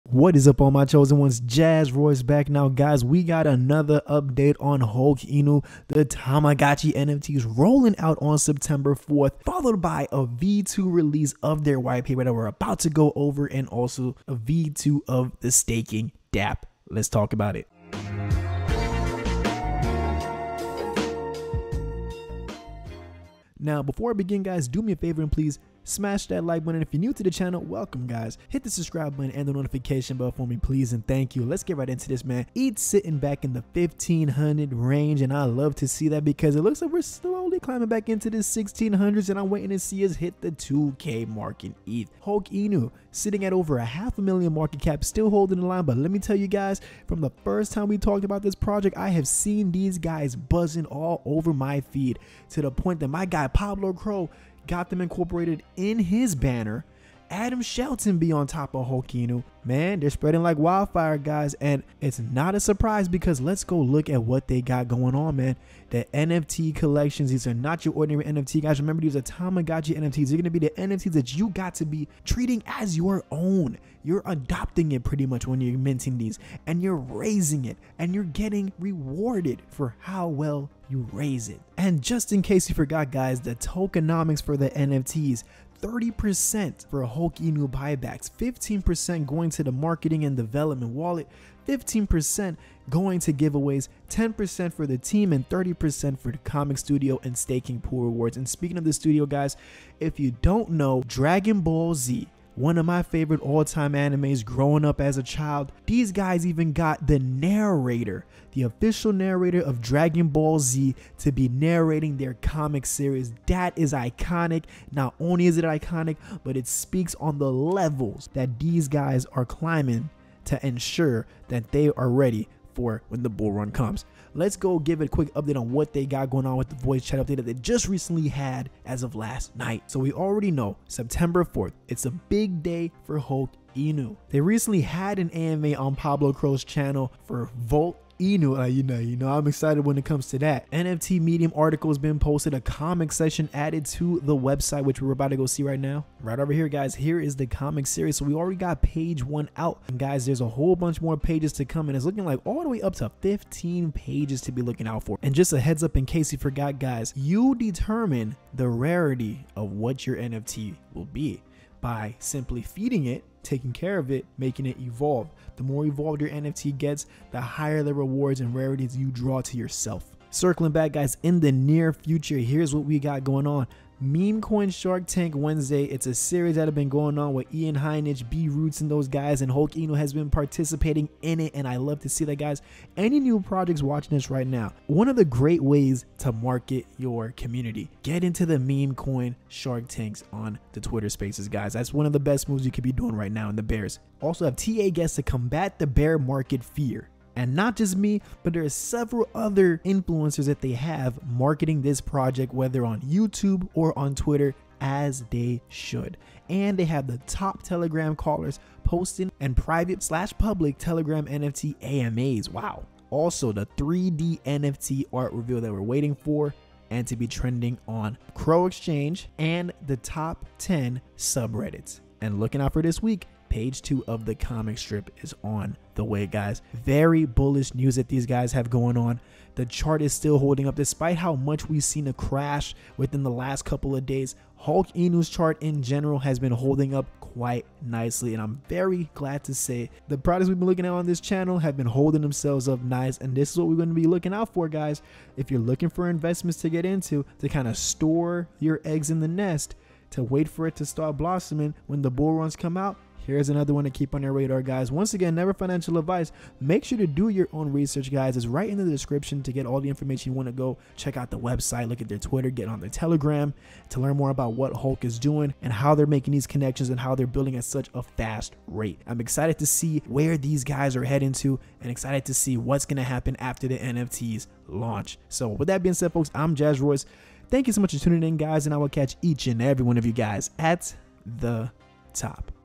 HULK INU TOKEN UPDATE: | NFTS LAUNCHING ON SEPT 4TH - V2 WHITEPAPER, STAKING & MORE!
**IM NOT A FINANCIAL ADVISOR**